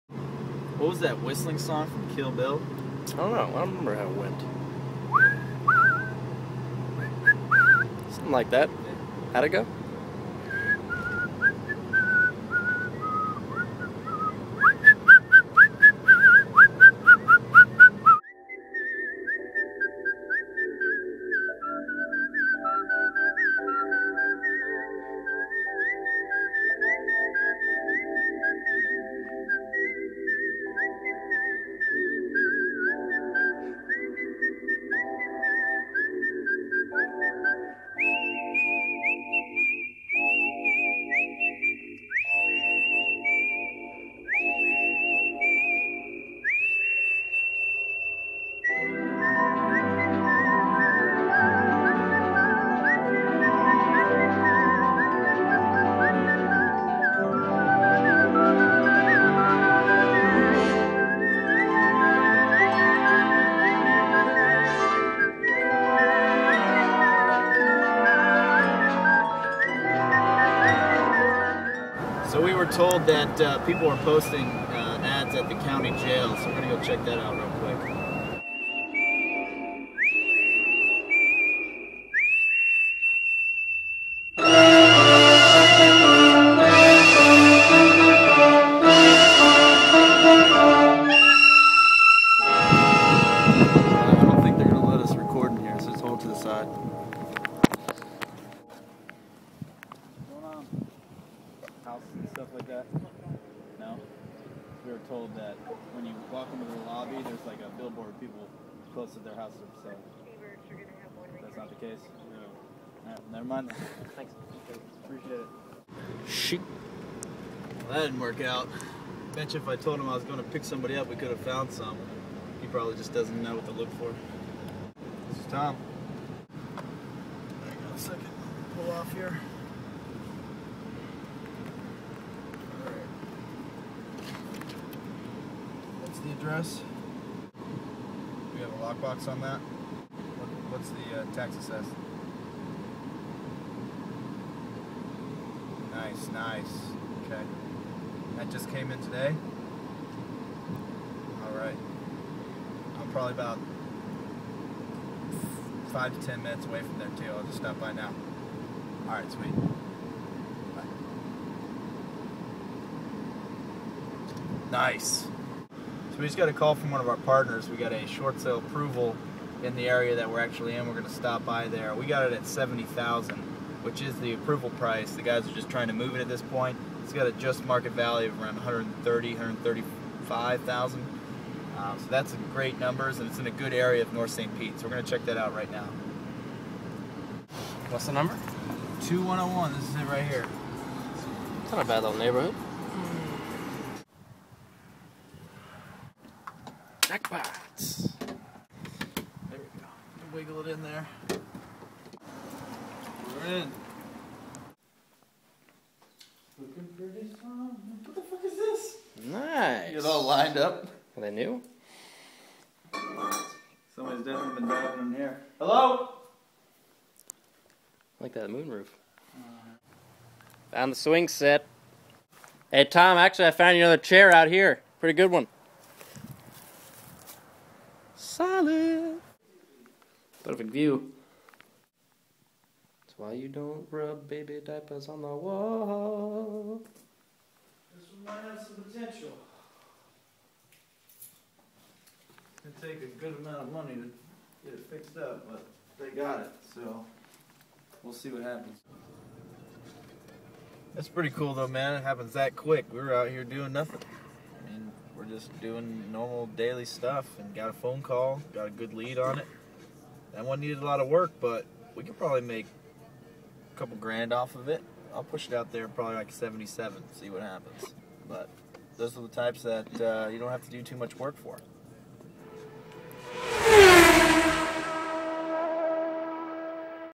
How to inspect a Short Sale! How to get good Shortsale deals!
Watch Tom Bell of Turbo Charged Real Estate show you how to make money with real estate by flipping houses! How to inspect a Short Sale! How to get good Shortsale deals!